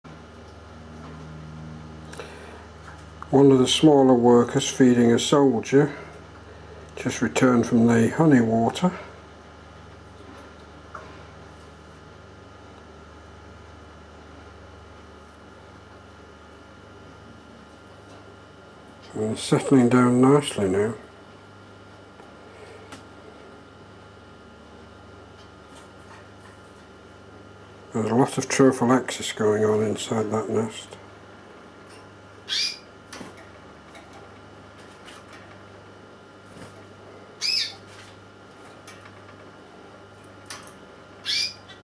Workers feed the queens
Our new colony of Pheidole yeensis showing the workers feeding the queens and soldiers via trophallaxis.